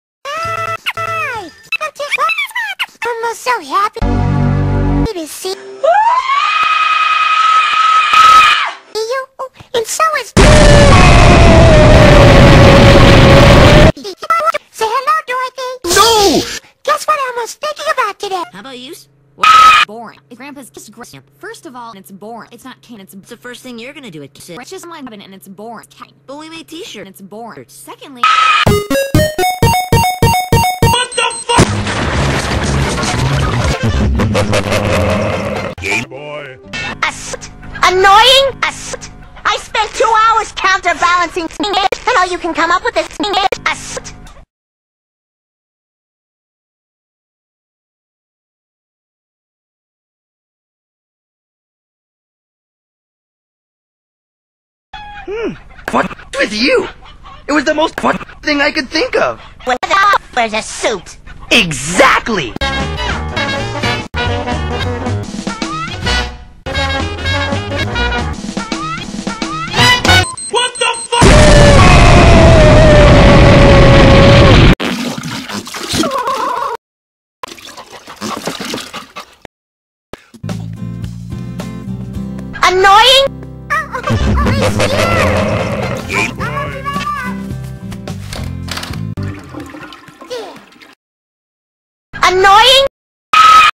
Youtube Poop Collab Entry By 017149
Basically a Recycled poop I made with other peoples poops, and my poops as well and some unreleased stuff I made a few years ago and yes you guessed it, I pooped it too.